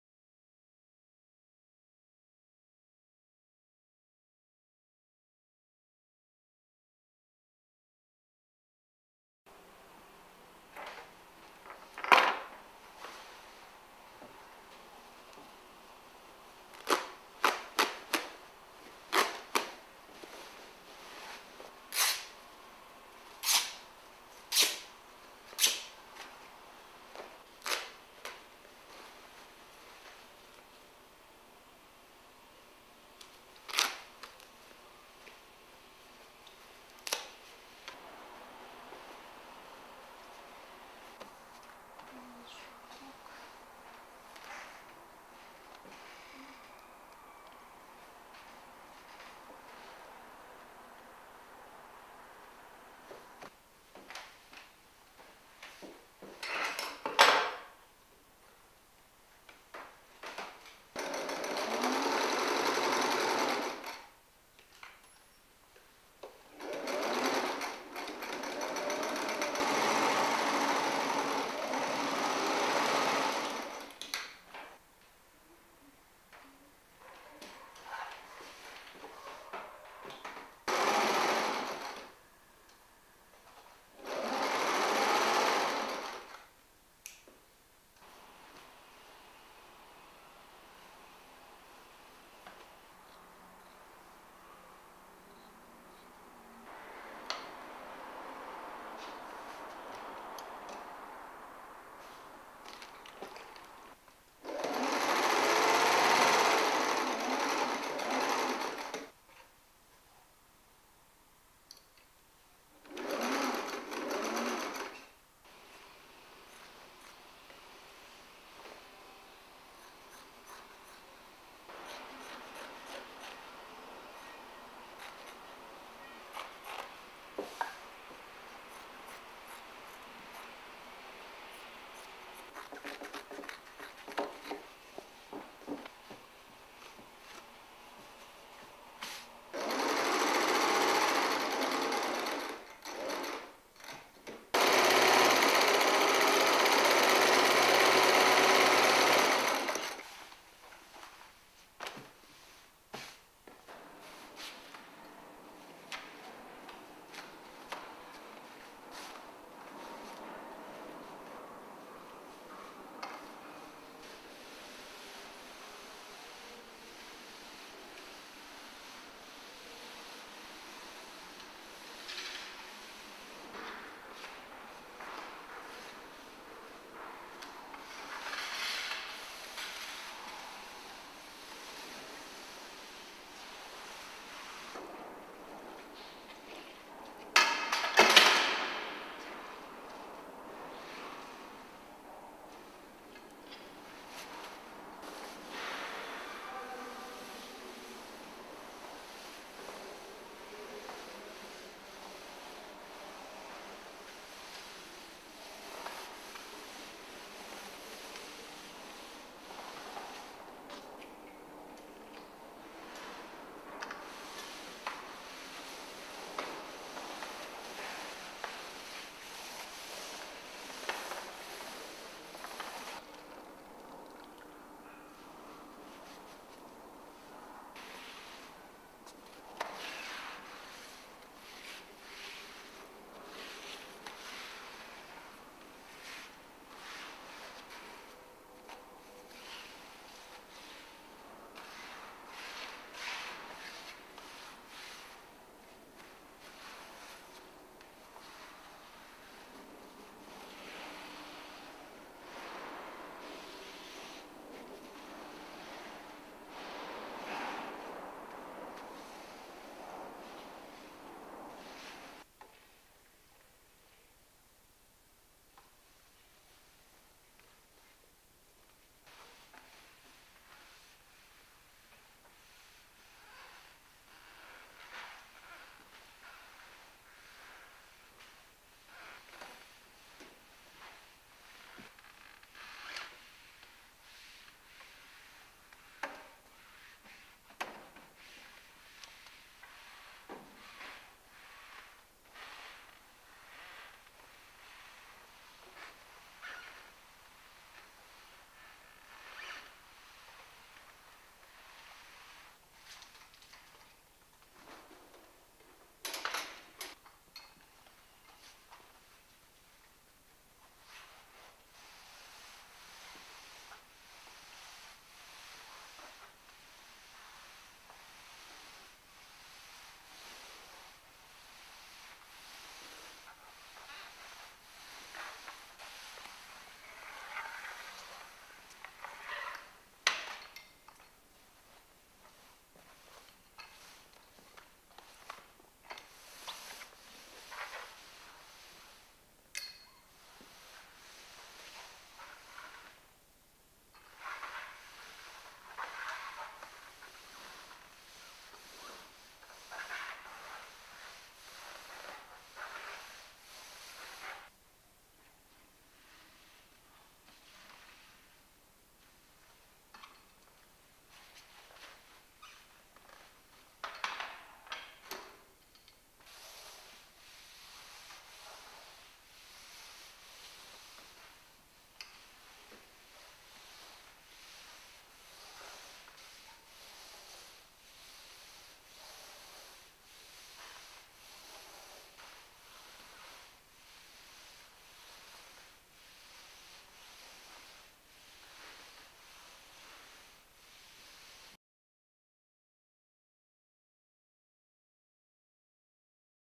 Cleaning up the city hall by using politician's clothes
koki tanaka, 2008

title: Cleaning up the city hall by using politician's clothes
cooperation: the University of Applied Arts Vienna and quartier21
other informations:
Location: Rathaus (The City hall of Vienna)